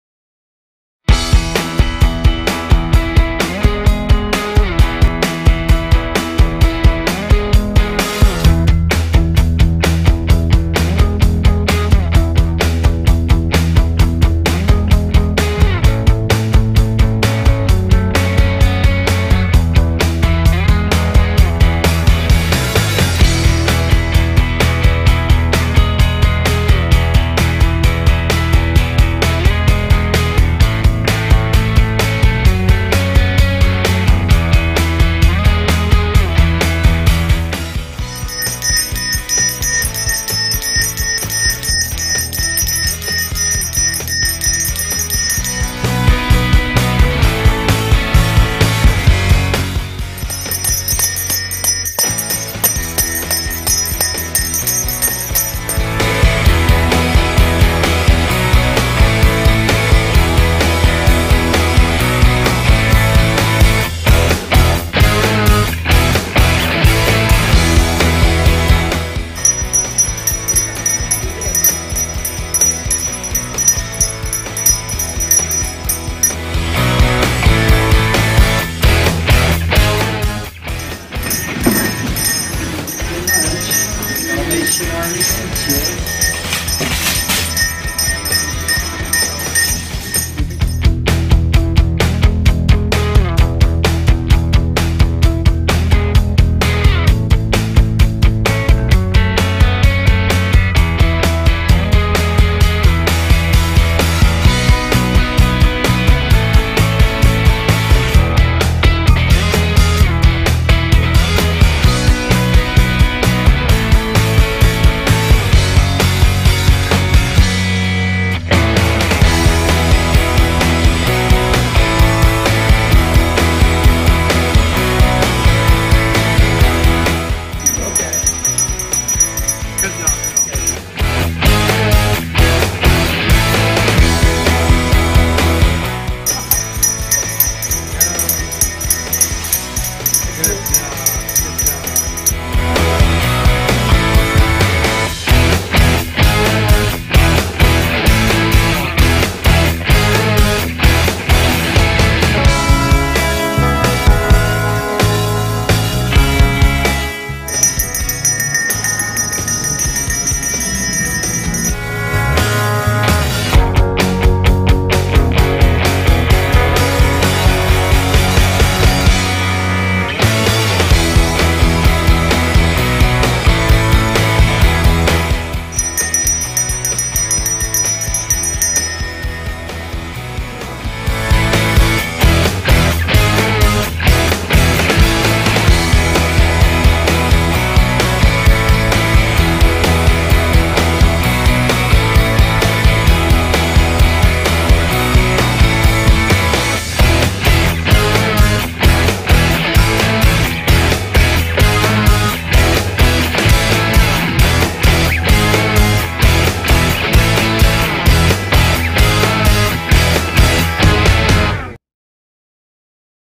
New Albany Rotary Bell ringing video
New Albany Rotary and the Salvation Army teamed up.  New Albany Rotary Club rang the bell for three days at the Kroger in New Albany.  Here's a few clips of our favorite moments.